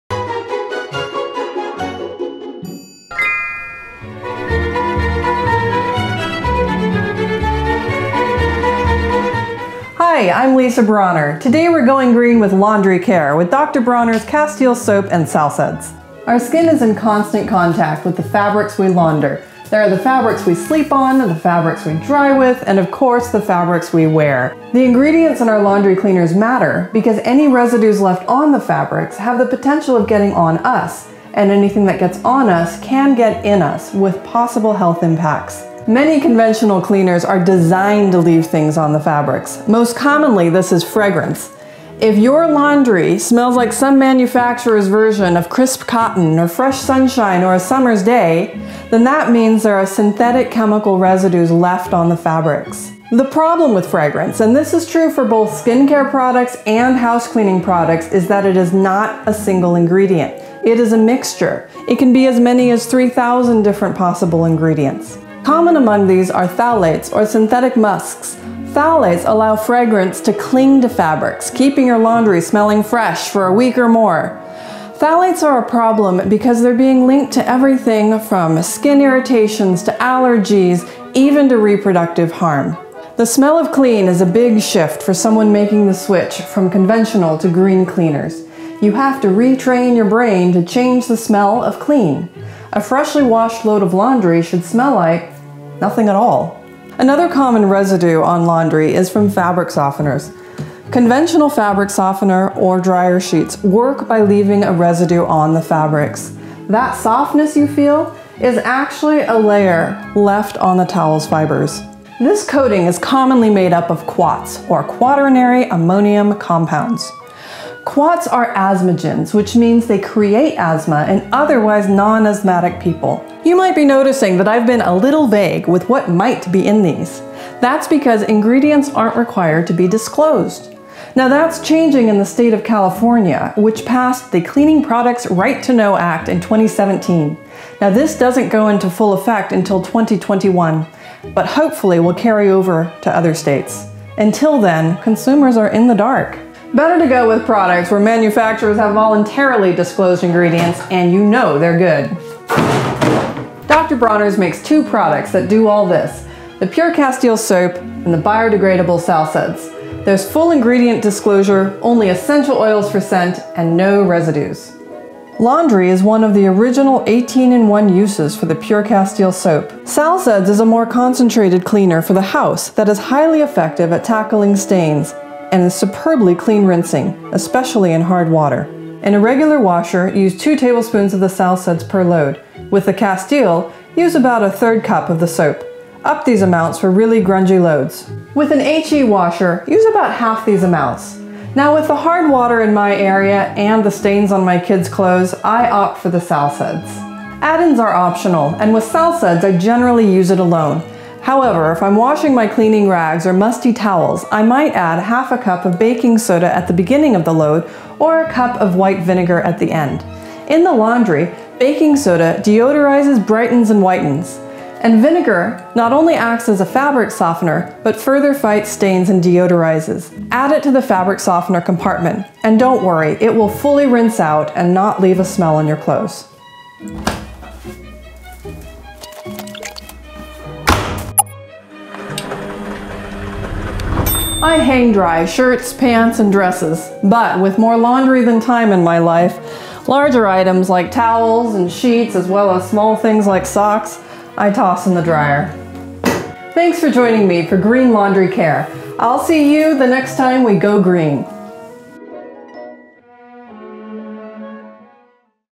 Green Laundry Care with Dr. Bronner’s
My laundry regimen is simple. For clothing, I use about 2-3 Tbsp. (30-45 mL) of Sal Suds or 1/3-1/2 c. (80-120 mL) of Pure-Castile Liquid soap for each large load in my regular washer. Halve these amounts in an HE washer.

When I’m washing something grubbier, like towels, I might throw in a ½ c. (120 mL) of baking soda and with the Castile, because I have hard water, I’ll add 1 cup (240 mL) of vinegar to the rinse water via the fabric softener compartment. Again, for an HE washing machine, halve these amounts. Both the Sal Suds and Castile Soap biodegrade readily and are safe for septic and greywater systems.

For the inevitable ketchup/grass/last night’s dinner on clothing, I pre-treat the stain by dabbing a small amount of Sal Suds directly on to it before washing. For broader stains, like ring-around-the-collar, I spray them with my Sal Suds All-Purpose Spray.

Then comes the drying. Drying our clothes is probably the single harshest thing we do to them, or at least it is once we’re old enough to find that sliding on our knees on asphalt is no longer fun. The dryer shortens the life-span of our clothes, not to mention takes a tremendous amount of energy. Air-drying clothes is best for the clothes, best for your energy bills, and best for the environment.

My laundry cabinet has both Sal Suds and Castile Soap, but for laundry I reach for Sal Suds more often because it’s a bit tougher on stains and more effective in my hard water. I got started using Sal Suds on my clothes not because my dad invented it (sorry, Pop), but because as I learned about problematic ingredients in cleaning products, I realized that any of them left on fabrics in the wash were going to spend a whole lot of time in contact with my skin, and my family’s skin. That adds up to a lot of exposure. In the video, I go into greater detail about these ingredients, but the end result was my wanting “clean” laundry cleaners, which the Pure-Castile soap and Sal Suds are.

Sal Suds cleaner shows greater than 60% biodegradation after 28 days per ISO 14593.